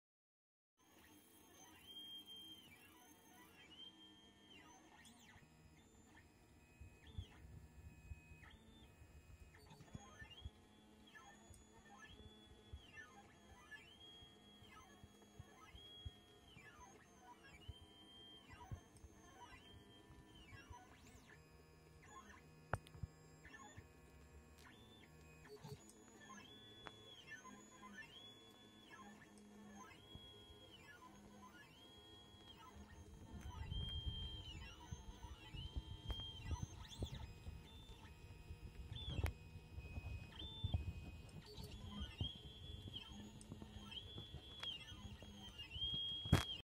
3D Printer Tronxy X1
Moai Statue 3D Printing on a Tronxy X1